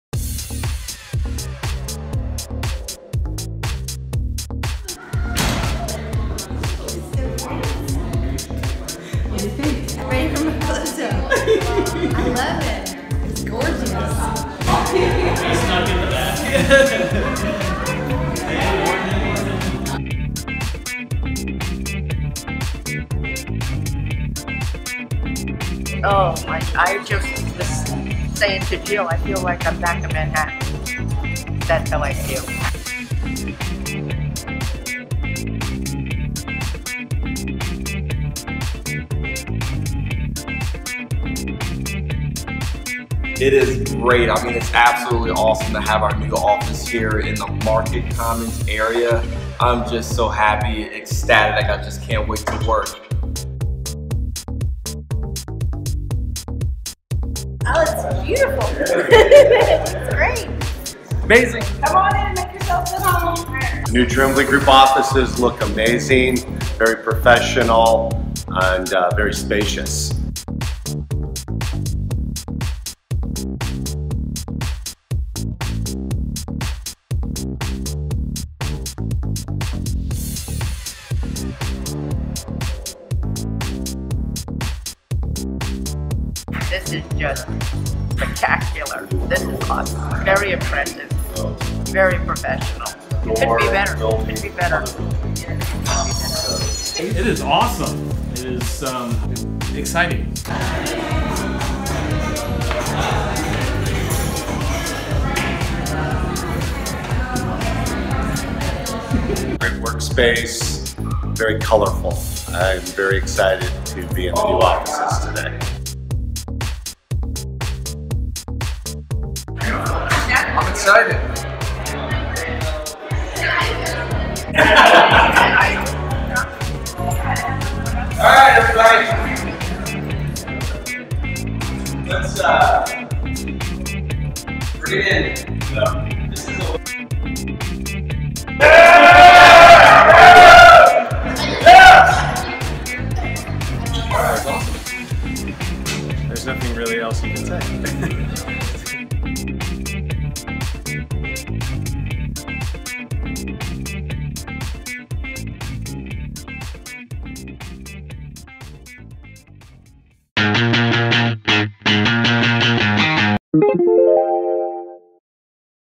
First Day Agent Reactions To Our New Office Space in The Market Common!
👉This week we opened our new offices in The Market Common and surprised our team! Then we topped the morning off with an engaging role play .. the way we 'role' at The Trembley Group. 🤩These are the reactions of our agents to our new space!